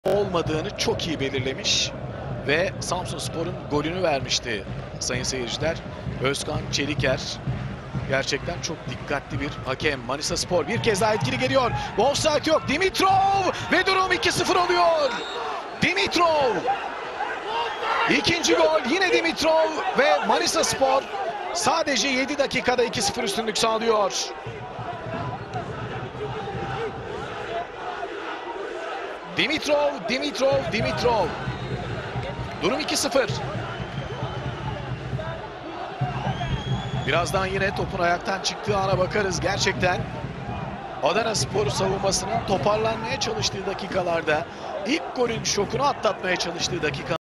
Gökhan Sazdağı 'nın Süper Asisti - Adanaspor maçı
Genç oyuncu Gökhan Sazdağı 'nın Adana maçındaki dribblingi ve muhteşem asisti